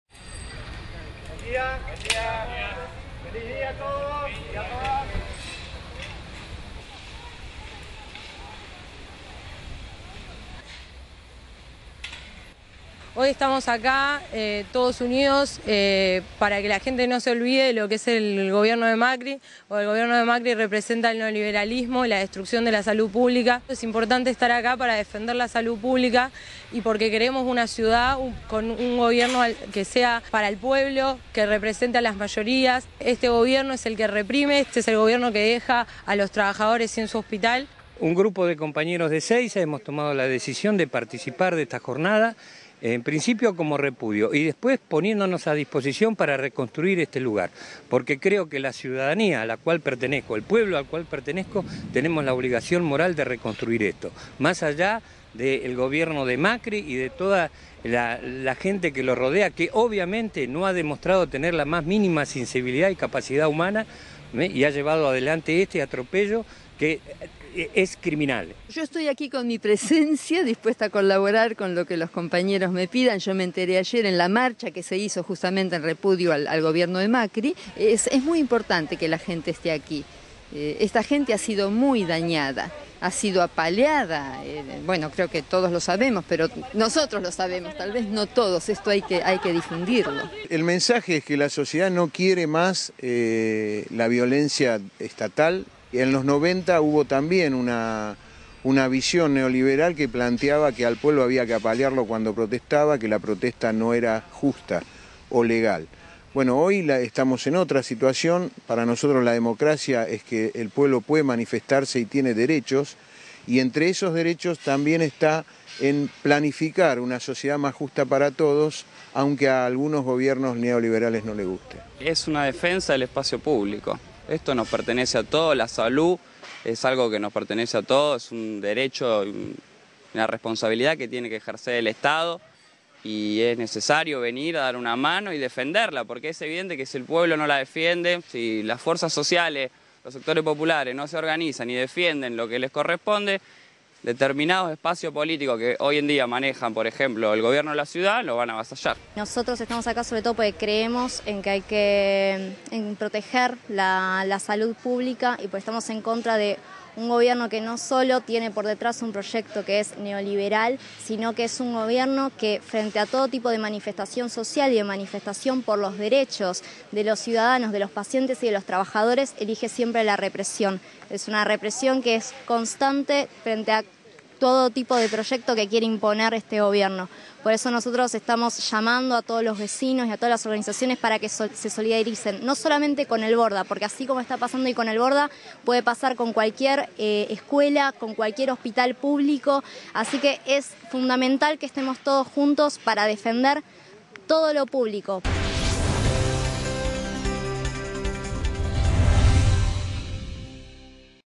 Jornada solidaria en el Hospital Borda
Trabajadores y trabajadoras del Hospital Borda junto a integrantes de organizaciones sociales, políticas y ciudadanía en general, participan de la jornada solidaria de limpieza y reconstrucción de los talleres protegidos destruidos el pasado viernes en el marco de la represión de la policía Metropolitana.